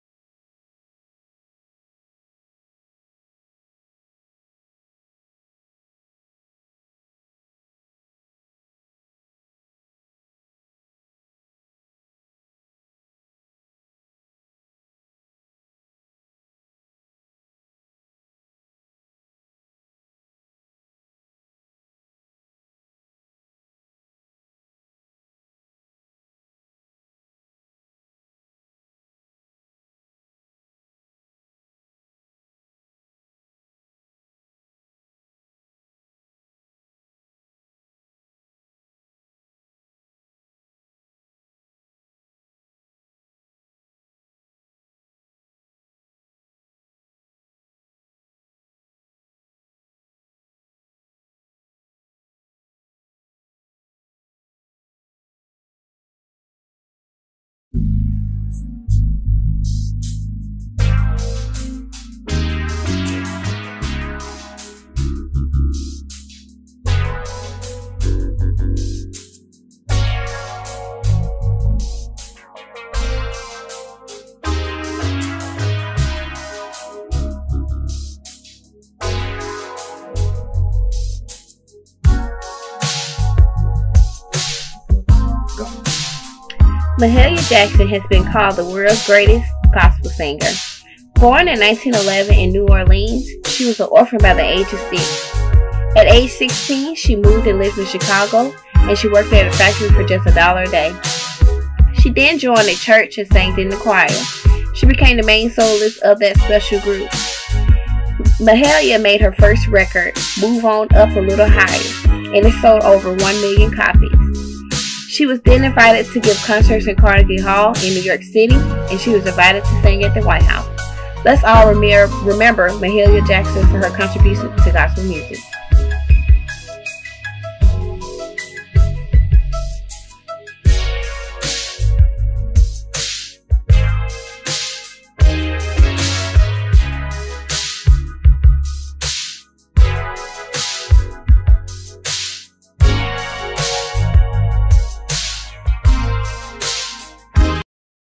BlackHistoryMonth Minute MahaliaJackson February 13, 2019
Legendary Gospel Singer mahaliaJackson is profiled in this BlackHistoryMonth Minute.